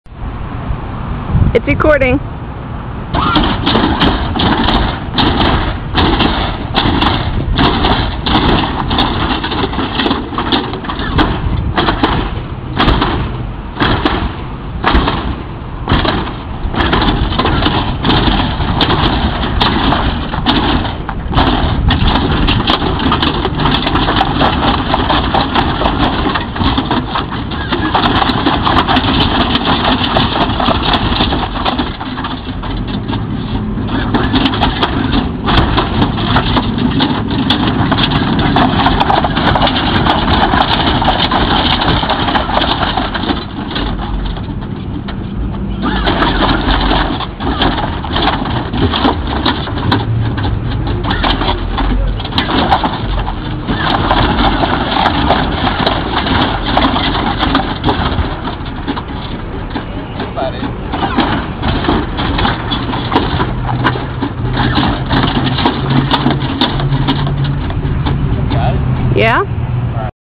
Dancing the Grand Prix at the supermarket
We got bored and decided to take the car up to the supermarket parking lot.